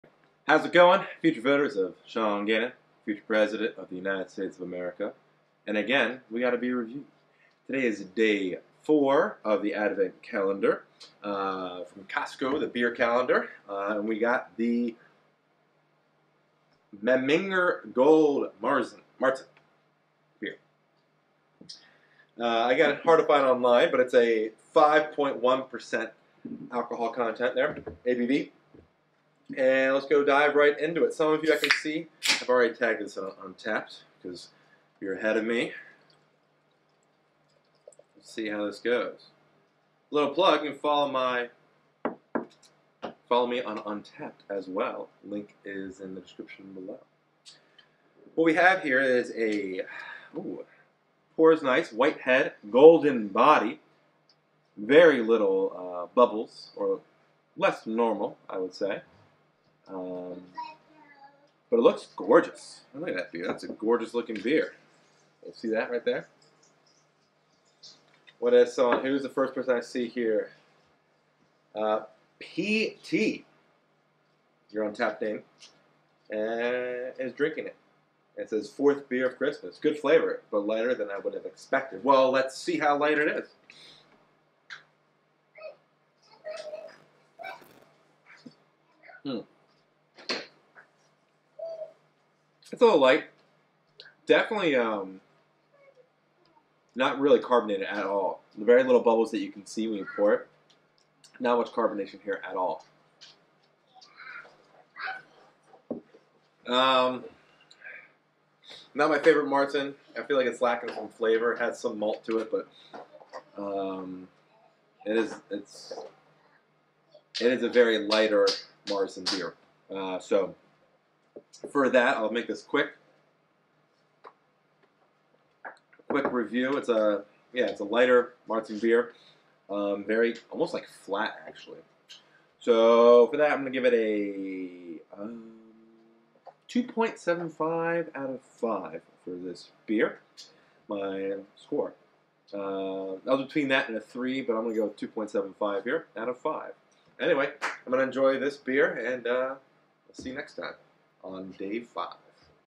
Memminger Brauerei by Dortmunder | Beer Review | 4k
I review the 4th beer from the Costco Beer Advent Calendar. It is the Memminger Brauerei by Dortmunder.

Always drink responsibility. I was not paid to make this video.